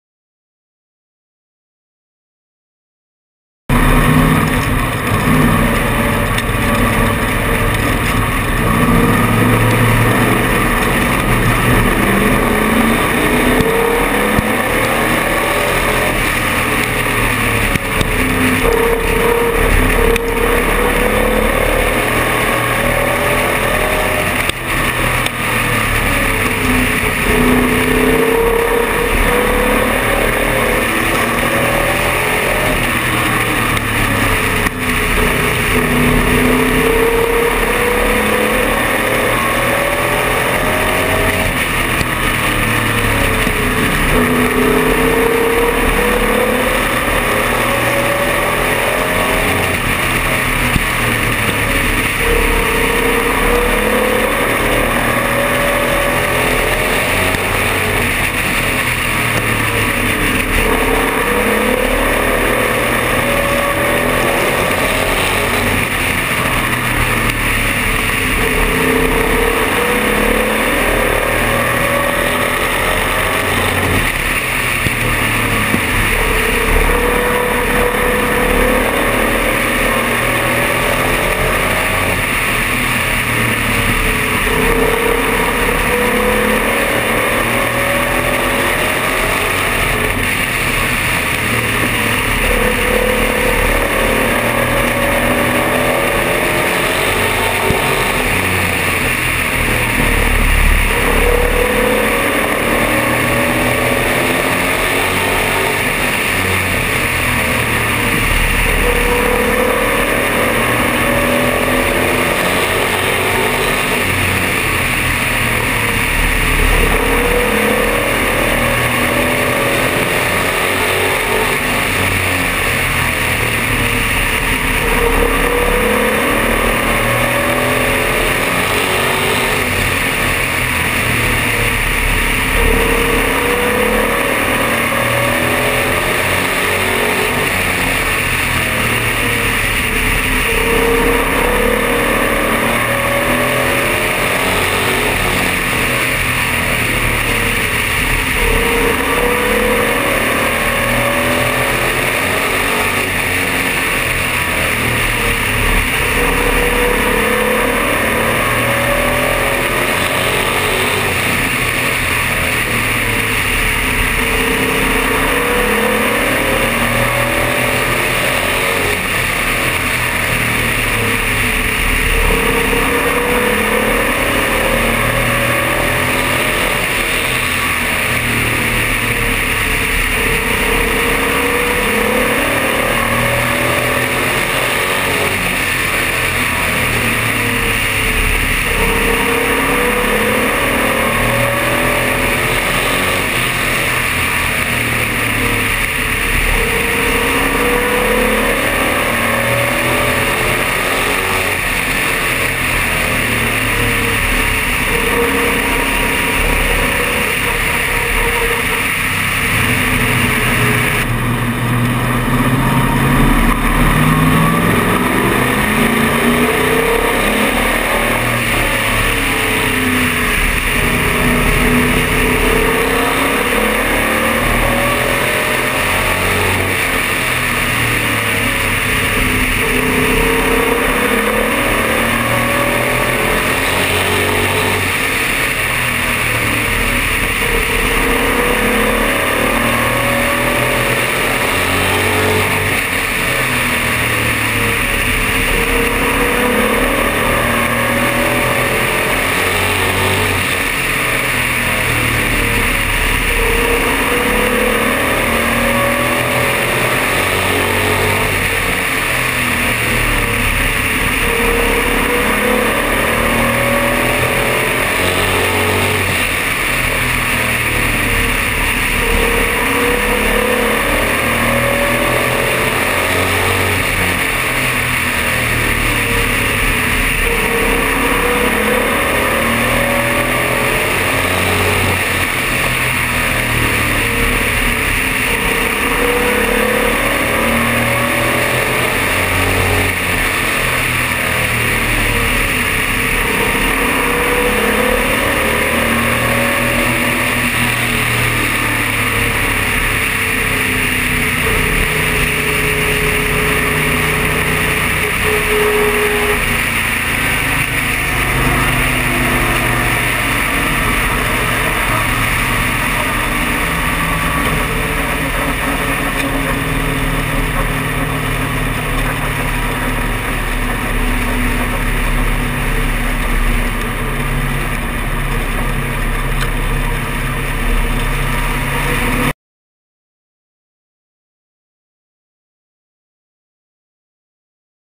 Sportsman Heat #2
Onboard footage of the second Sportsman heat race from Neil Schneider's #93 RIchard Reimer Reality/Burco Electric Chevy Camaro.

Recorded on June 15th/2019 at Wyant Group Raceway in Saskatoon.